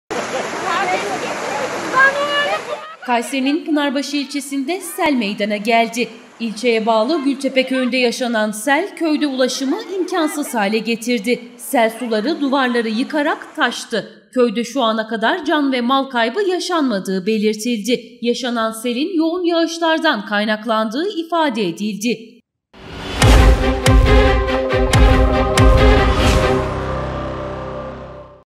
Pınarbaşı'da Su Baskını
KAYSERİ,(BGC)-Kayseri'nin Pınarbaşı ilçesinde sel meydana geldi.

İlçeye bağlı Gültepe köyünde yaşanan sel, köyde ulaşımı imkansız hâle getirdi. Sel suları, bazı duvarları da yıkarak taştı. Köyde şu ana kadar can ve mal kaybı yaşanmadığı belirtildi. Yaşanan selin yağışlardan kaynaklandığı ifade edildi.